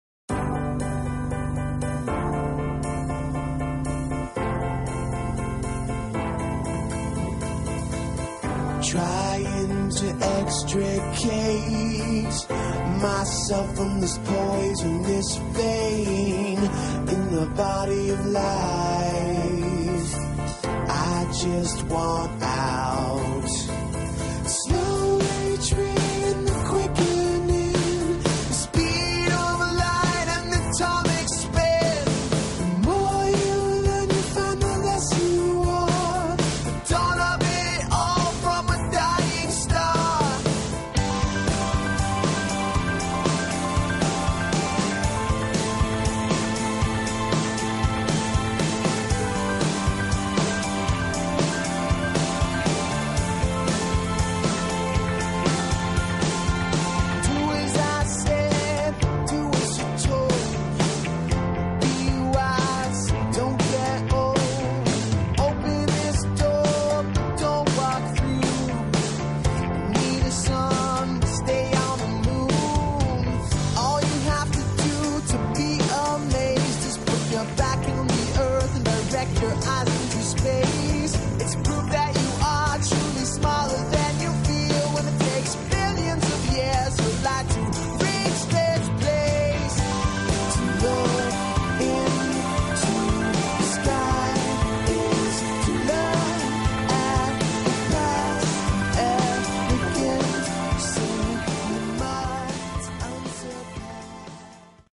Darwin Week 2011
Promotional video for Darwin Week 2011!  Hosted by the UNI Freethinkers & Inquirers at the University of Northern Iowa.